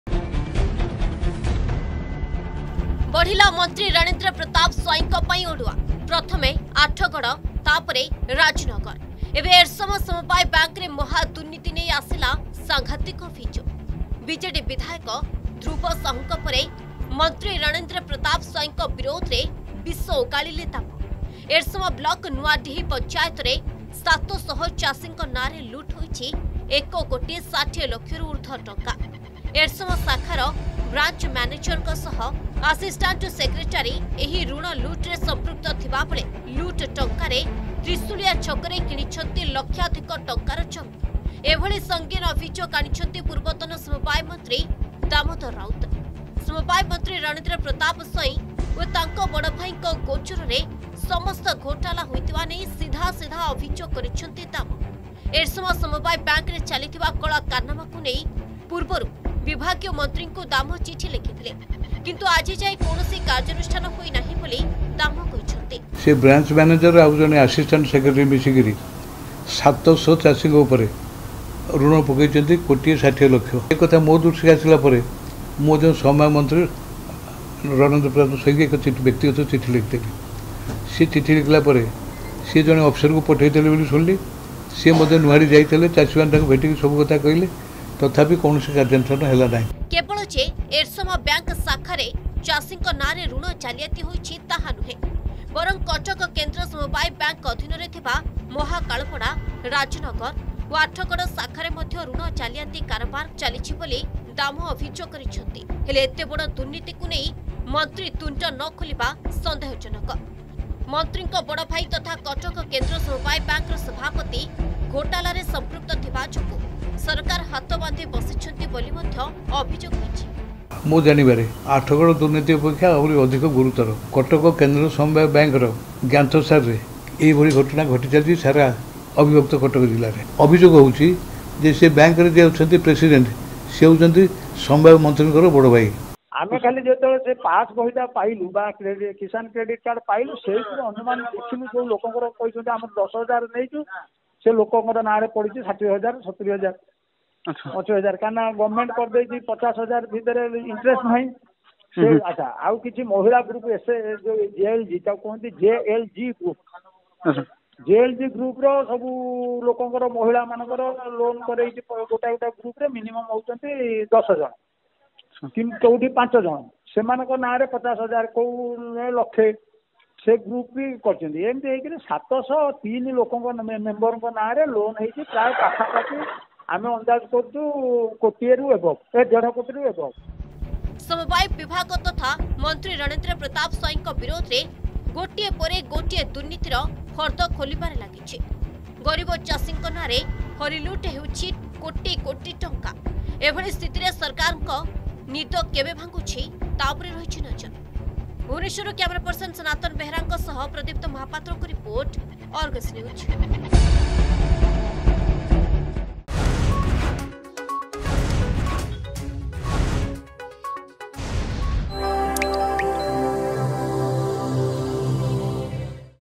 ବଢିଲା ମନ୍ତ୍ରୀ ରଣେନ୍ଦ୍ର ପ୍ରତାପ ସ୍ଵାଇଁଙ୍କ ପାଇଁ ଅଡୁଆ
ବଢିଲା ମନ୍ତ୍ରୀ ରଣେନ୍ଦ୍ର ପ୍ରତାପ ସ୍ଵାଇଁଙ୍କ ପାଇଁ ଅଡୁଆ । ପ୍ରଥମେ ଆଠଗଡ ତାପରେ ରାଜନଗର । ଏବେ  ଏରସମା ସମବାୟ ବ୍ୟାଙ୍କରେ ମହାଦୁର୍ନୀତି ନେଇ ଆସିଲା ସାଙ୍ଘାତିକ ଅଭିଯୋଗ । ବିଜେଡି ବିଧାୟକ ଧ୍ରୁବ ସାହୁଙ୍କ ପରେ ମନ୍ତ୍ରୀ ରଣେନ୍ଦ୍ର ପ୍ରତାପ ସ୍ଵାଇଁଙ୍କ ବିରୋଧରେ ବିଷ ଓଗାଳିଲେ ଦାମ ।

Ranendra Pratap Swain